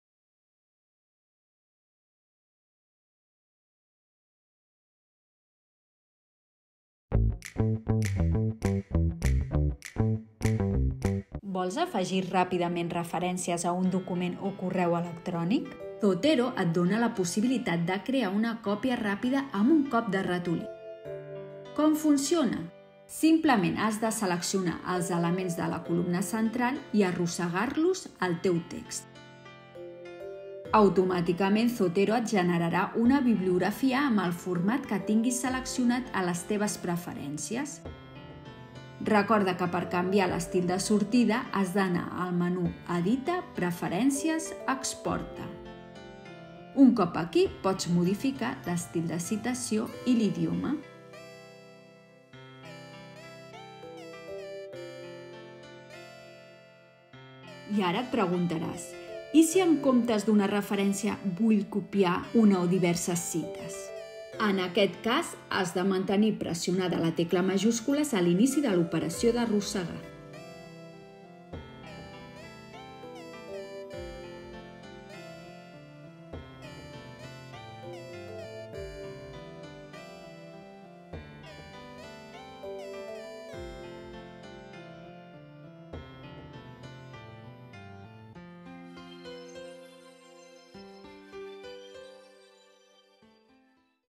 Zotero - Citacions ràpides
Vídeo on s'explica com crear citacions ràpides amb Zotero